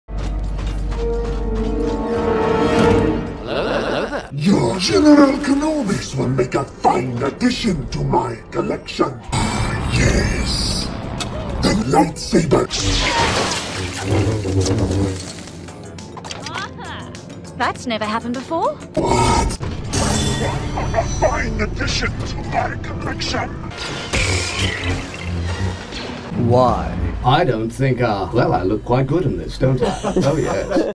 "General KENOBI..."
Hello there! Multiple Kenobi's approach General Grievous to say "Hello there!"
How will our Jedi General react?